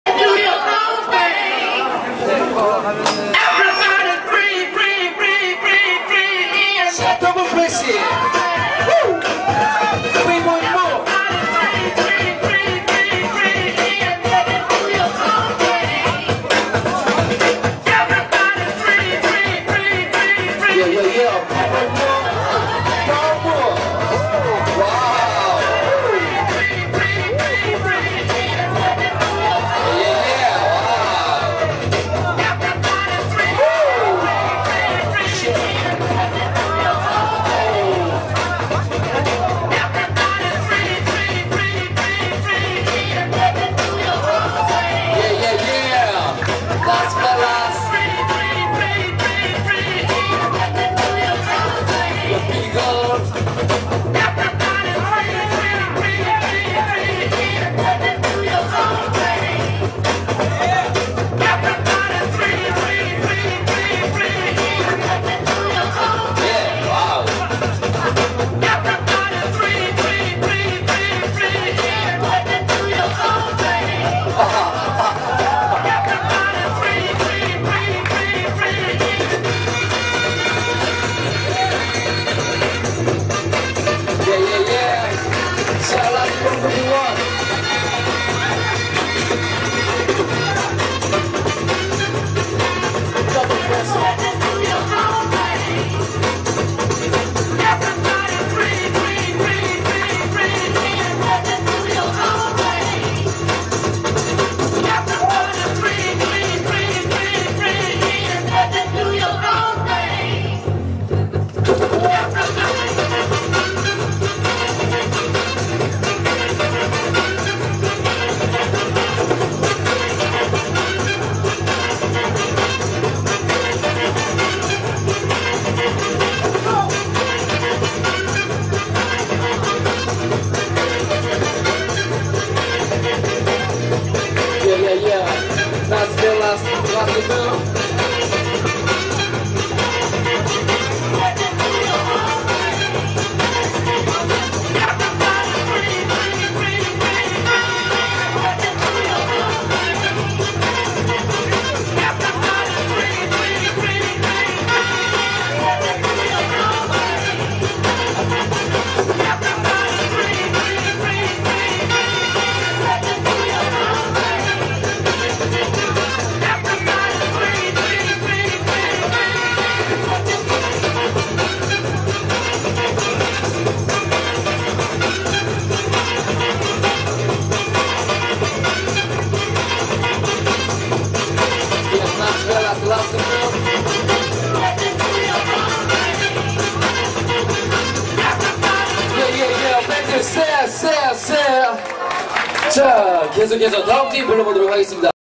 예선_20 20110129 LEADMOS VOL.5
예선전
Double Fresh (Mob & Ironman) VS

LEADMOS BBOY SESSION VOL. 5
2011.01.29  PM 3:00
@T.I.P STUDIO SEOUL